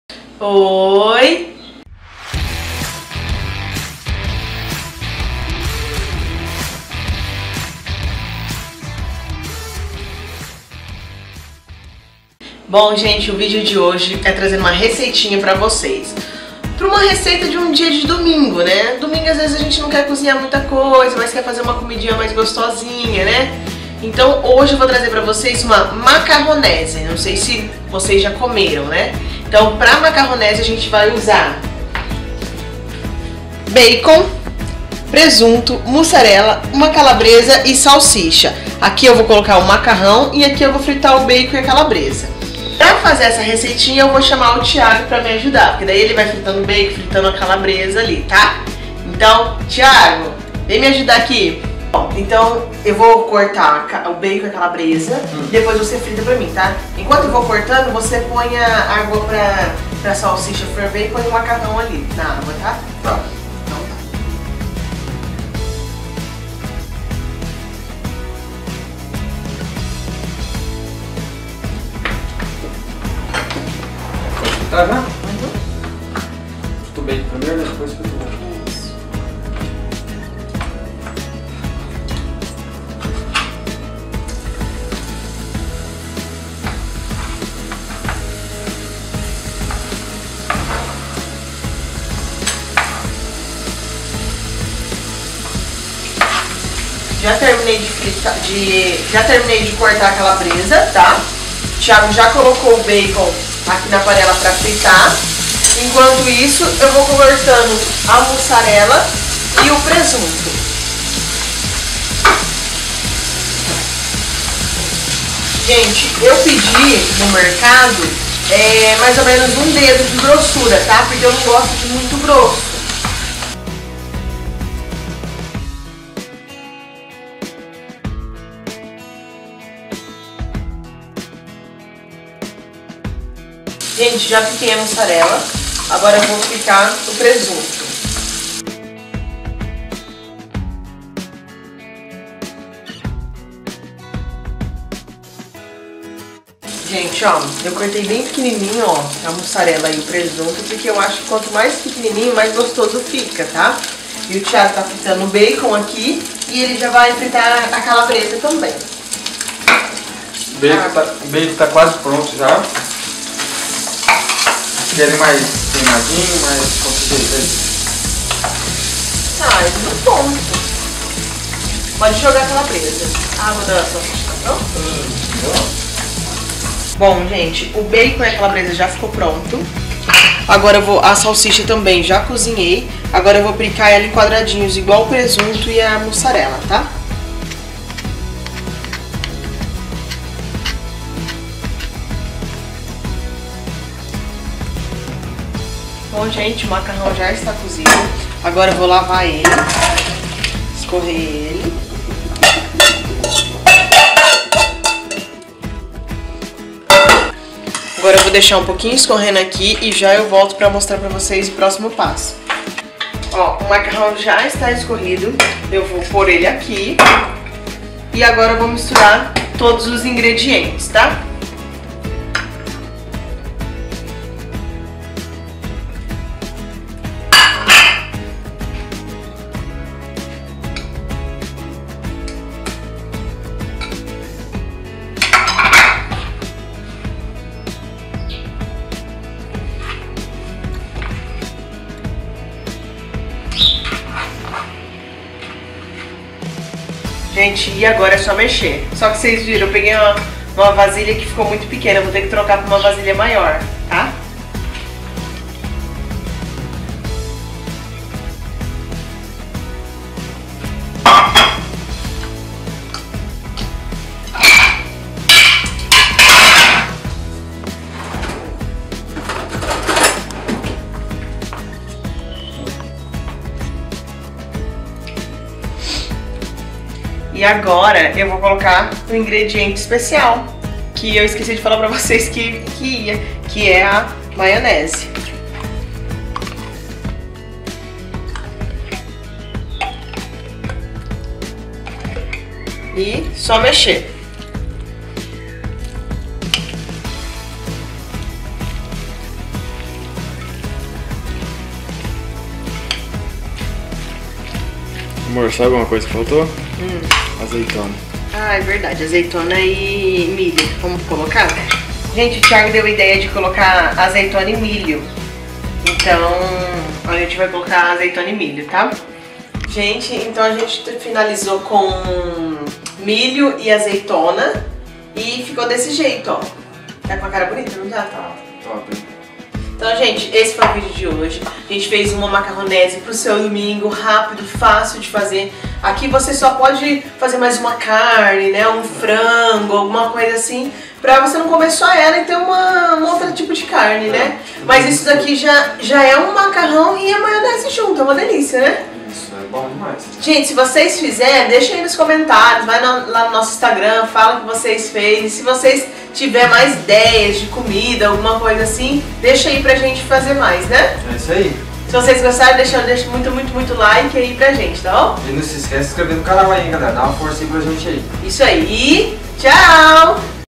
PRATO SIMPLES E FÁCIL PARA SEU DOMINGO ( MACARRONESE )
OLA PESSOAL
NESSE VIDEO IREI ENSINAR UMA RECEITA MUITO DELICIOSA
IREMOS FAZER UMA MACARRONESE  FICOU MUITO MARAVILHOSA ESSA RECEITA